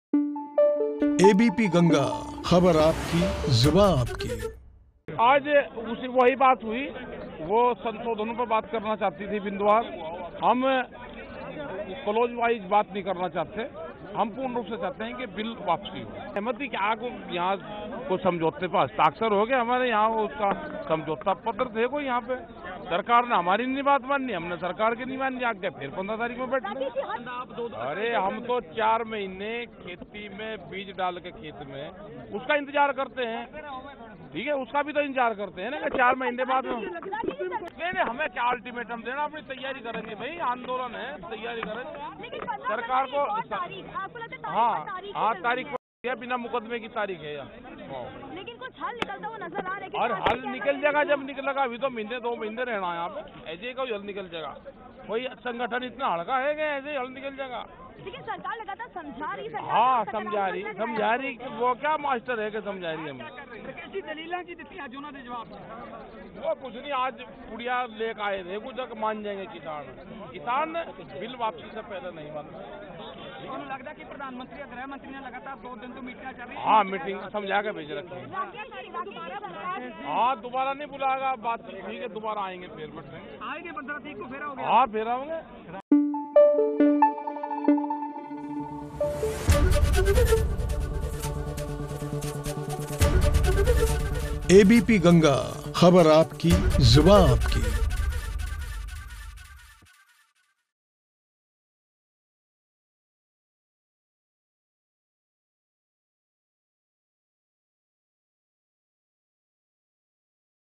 सरकार को हमारी बात नहीं माननी, हमने भी सरकार की बात नहीं माननी: Rakesh Tikait | Kisan Andolan
भारतीय किसान यूनियन के राष्ट्रीय प्रवक्ता राकेश टिकैत ने कहा कि तारीख पर तारीख चल रही है. बैठक में सभी किसान नेताओं ने एक आवाज़ में बिल रद्द करने की मांग की. हम चाहते हैं बिल वापस हो, सरकार चाहती है संशोधन हो. टिकैत ने कहा कि सरकार ने हमारी बात नहीं माननी, तो हमने भी सरकार की बात नहीं मानी.